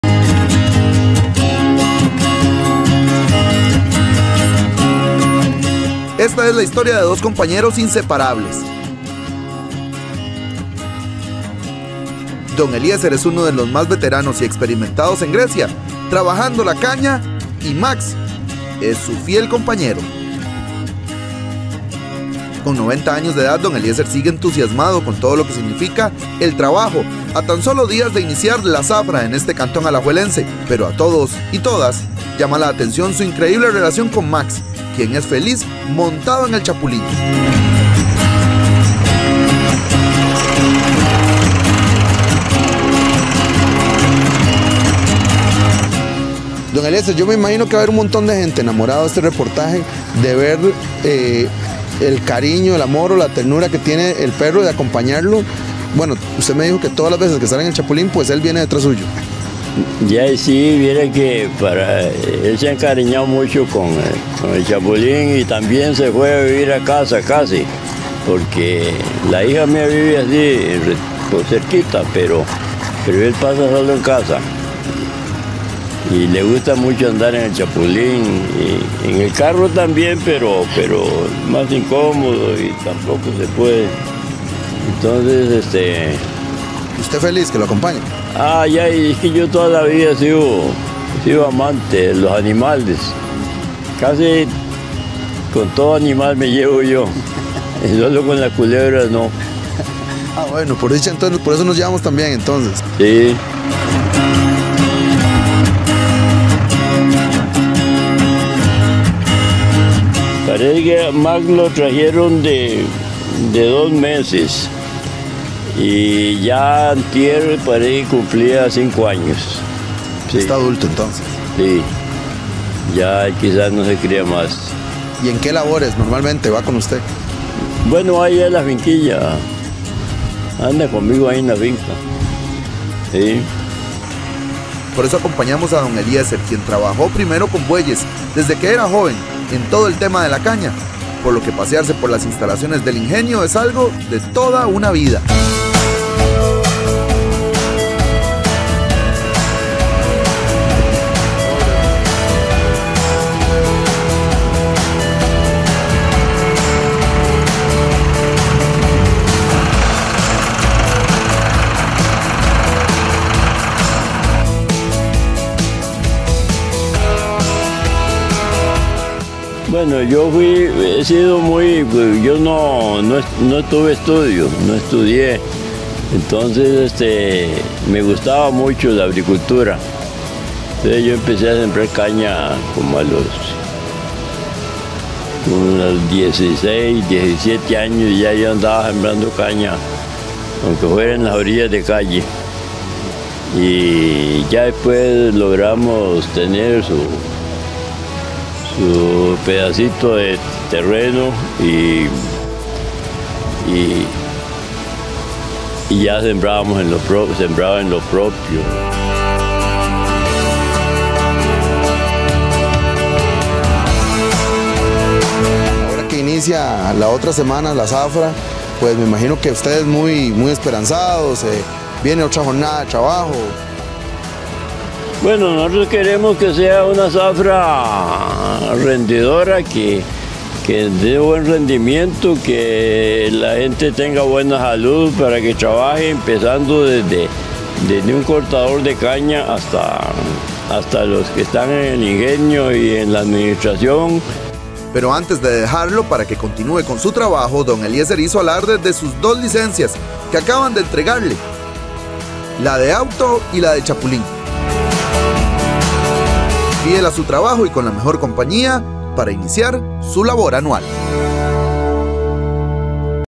¡Descubra la hermosa historia de don Eliecer y Max desde Grecia!
Son ejemplo de una amistad inseparable.

Es momento de conocer la historia de don Eliecer y Max; lo interesante es que siempre están juntos, pero hay quienes dicen que Max nunca sale sin la compañía de don Eliecer.